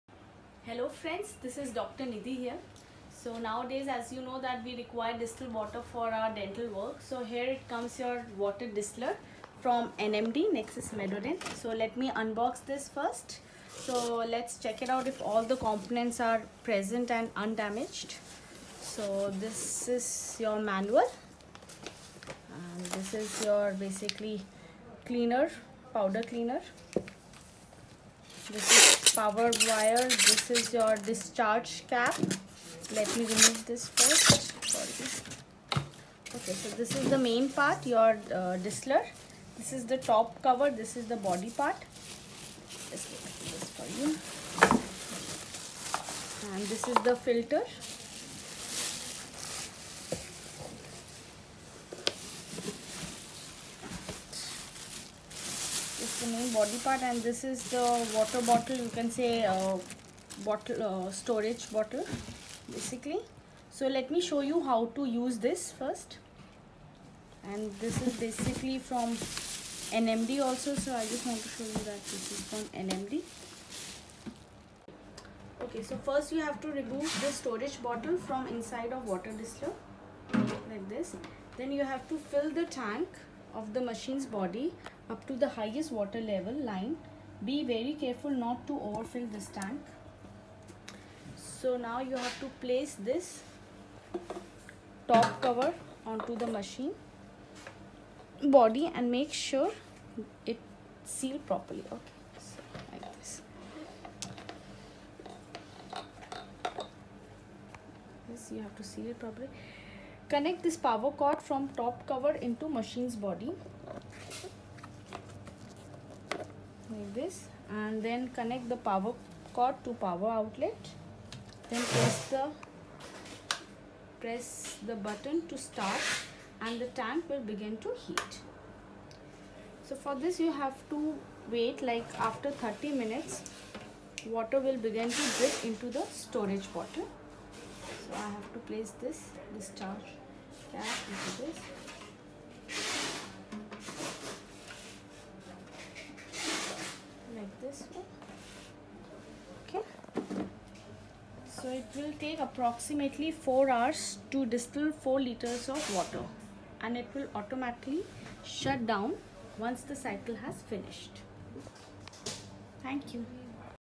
Water Distiller from Nexus Medodent ( NMD) Reviewed by Dr Nidhi Shah
High quality and robust build water Distiller by Nexus Medodent provides you unstoppable work at your clinic and gives a back end support to your all Autoclave and disinfection needs .